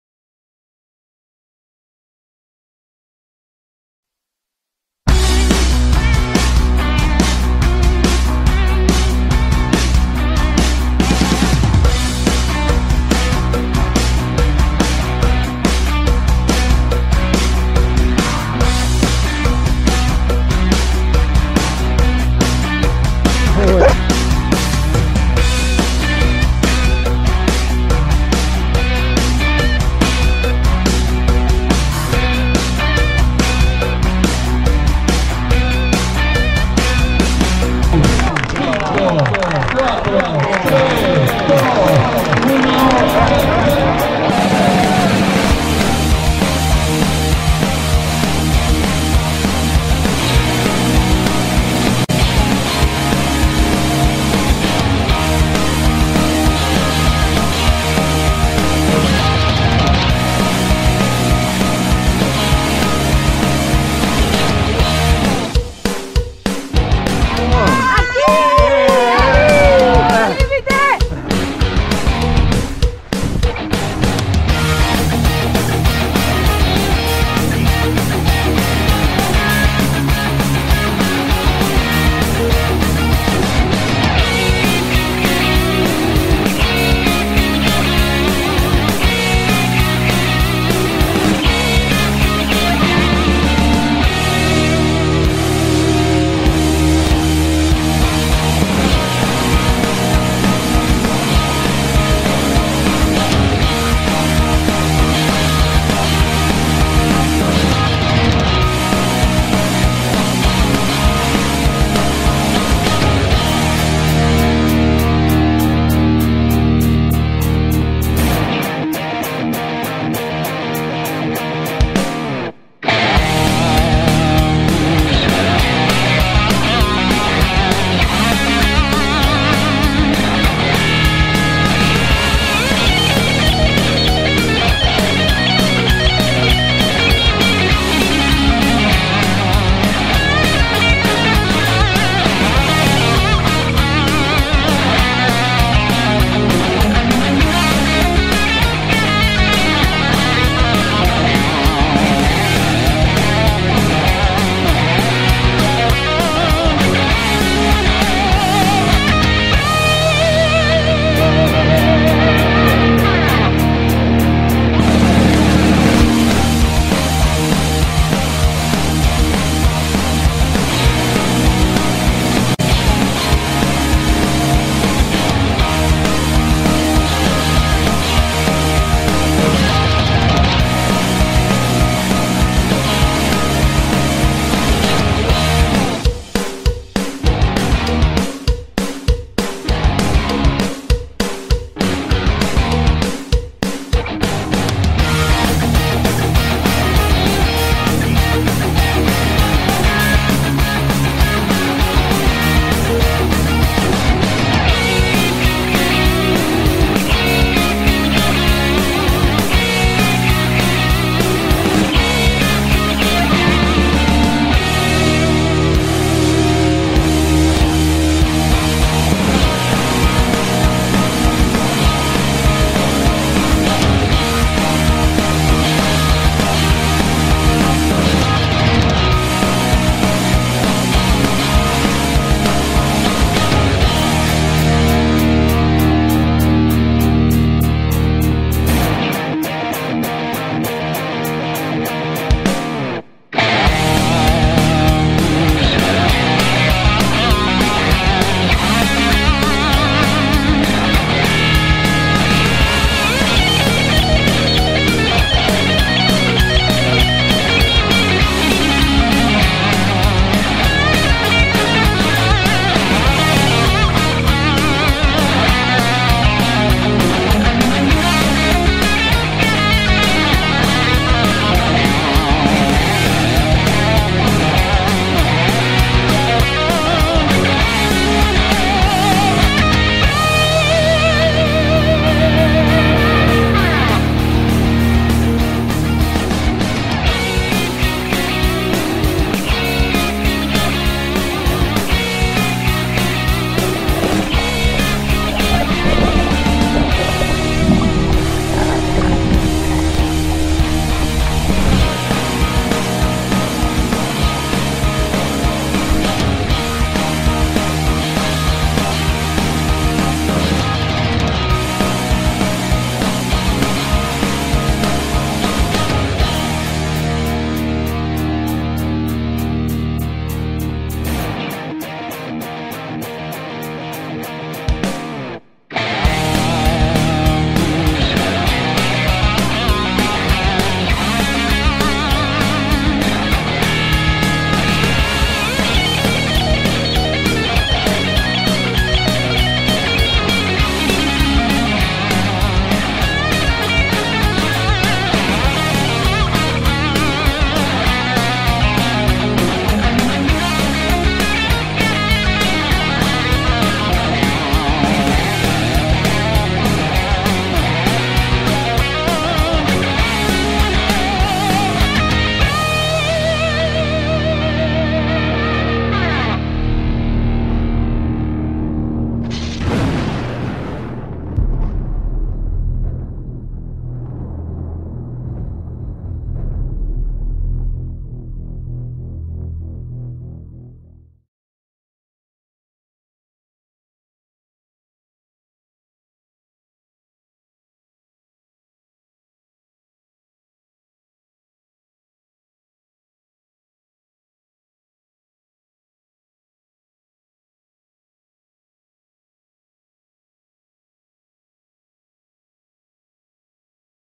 CAMPO AL LIMITE - SEGUNDA ETAPA - LA VANGUARDIA STA FE 2022
22 de mayo del 2022, segunda etapa del campeonato Campo al limite, esta vez en La Vanguardia, Santa Fe, con tres distancias de 8, 18 y 28 K. Organizado por Al Limite Aventuras.
En un dia que amaneció con mucha niebla, pero a la hora de la largada el sol brillo y estuvo así, durante toda la jornada.